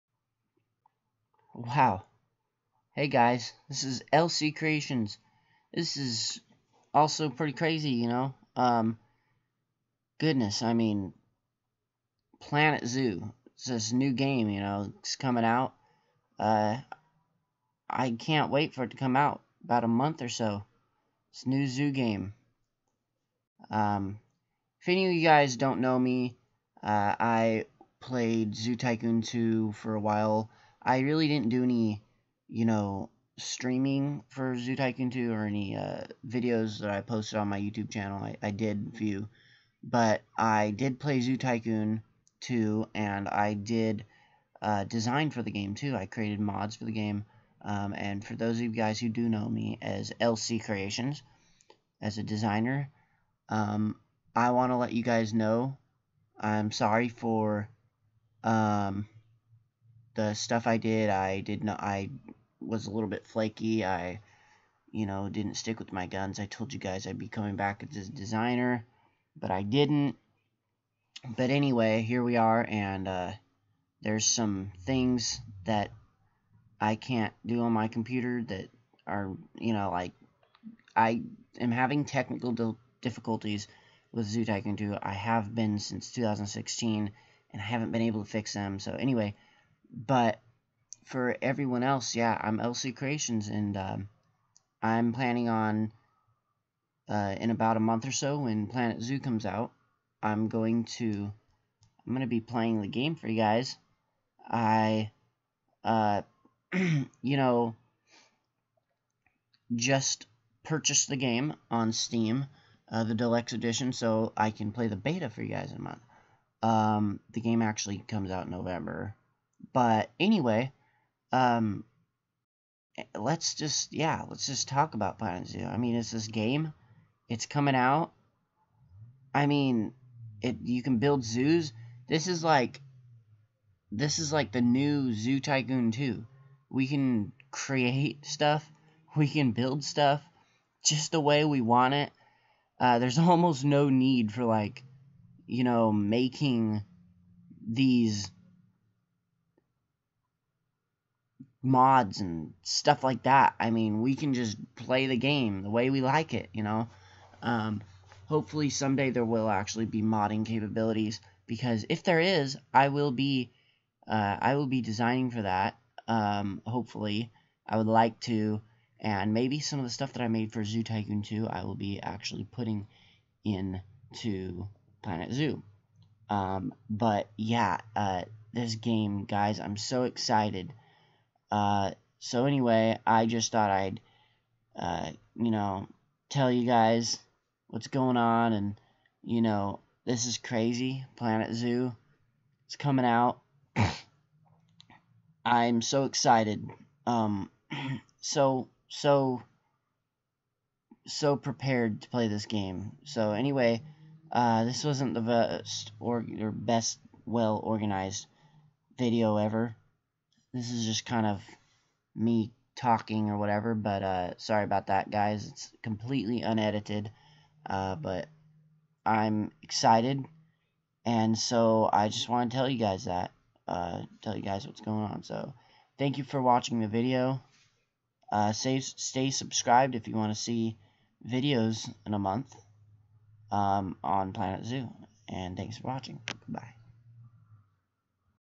Planet Zoo Is Comming
Just me talking about recent news about me and zoo games.